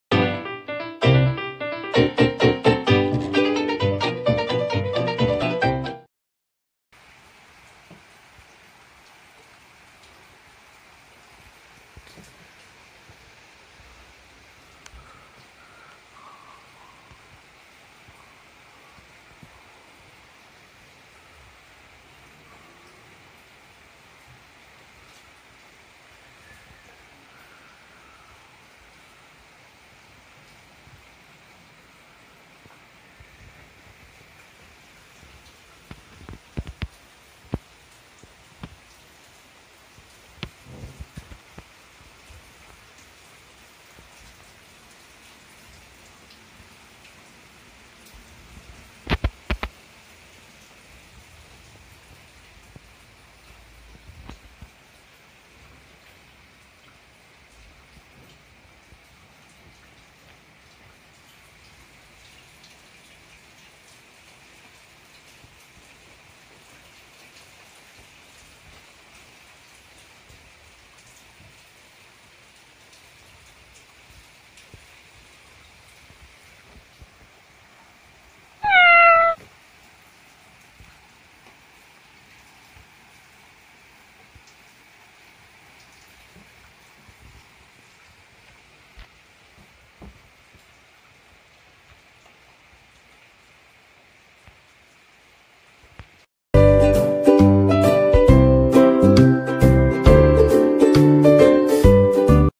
雨を見るナナちゃん猫
一日雨が降った日、ナナちゃんは二階から雨を見ました。